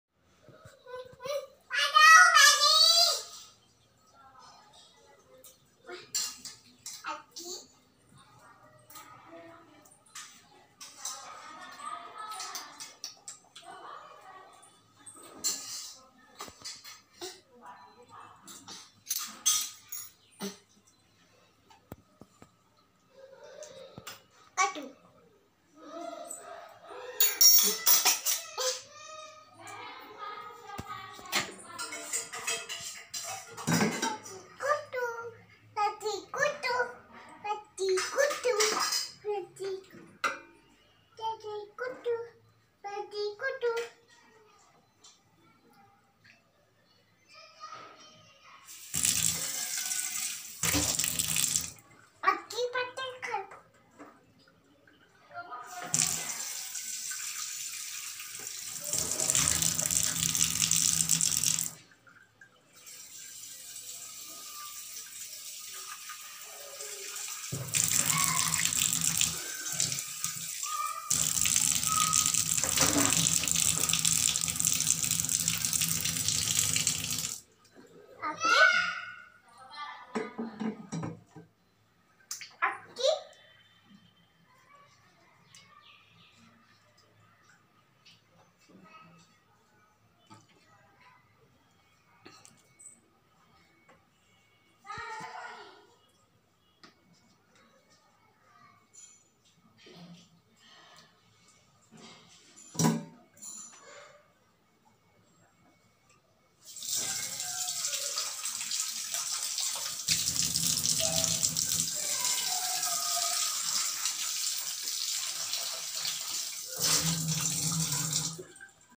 anvi is cooking food vlog
two year baby playing in kicten